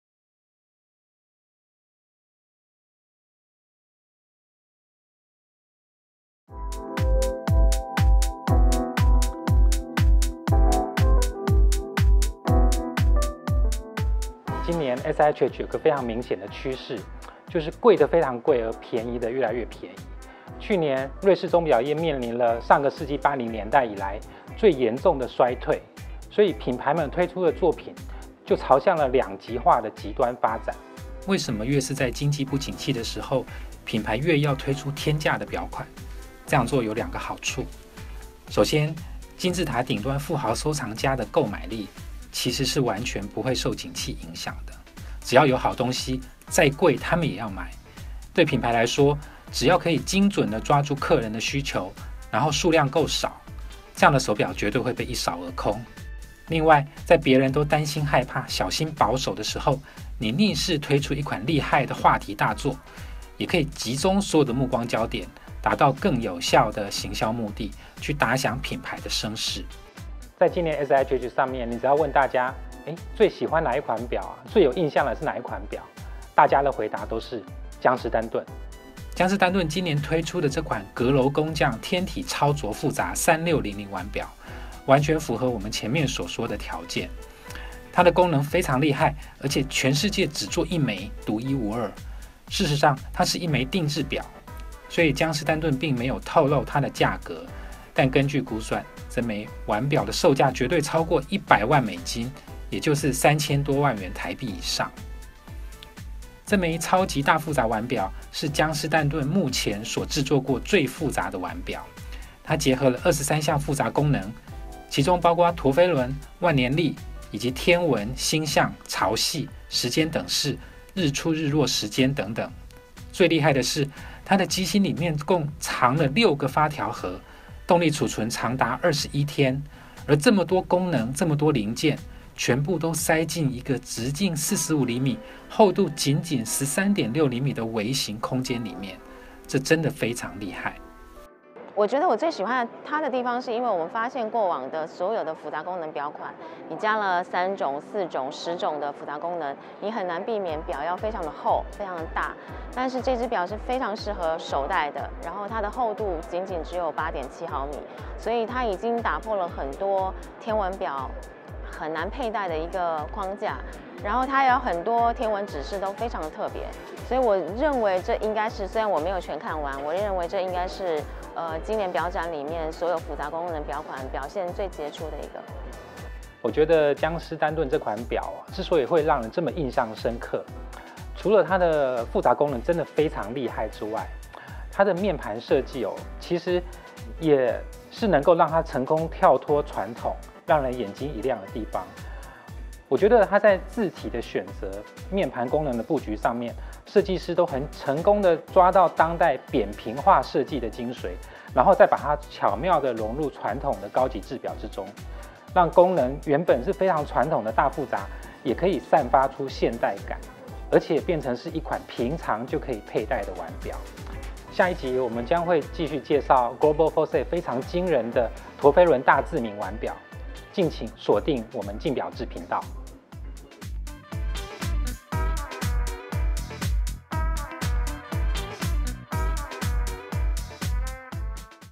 SIHH2017系列報導2：VACHERON CONSTANTIN 江詩丹頓大複雜天文錶
今年，SIHH有一個非常重要的趨勢。就是，貴的非常貴，而便宜的愈來愈便宜。去年，整個瑞士鐘錶業面臨了上世紀80年代以來最嚴重的衰退。在這樣嚴苛的環境之下，品牌推出的作品，朝向二極化的極端發展。
為什麼愈是在經濟不景氣的時候，品牌愈要推出天價的錶款？這樣做有二個好處。首先，金字塔頂端富豪收藏家的購買力，其實是完全不會受景氣影響的。只要有好東西，再貴他們也要買。對品牌來說，只要可以精選的抓住客人的需求，然後數量夠少。這樣的手錶絕對會被一掃而空，甚至你可能還要傷腦筋該分配給誰。
另外，在別人都擔心害怕、小心保守的時候，你逆勢推出一款厲害的話題大作，也可以集中所有的目光焦點，達到更有效的行銷目的，去打響品牌的聲勢。
在今年的SIHH上，你問大家，最喜歡哪一款錶，最有印象是哪一款錶？大家都會回答：江詩丹頓。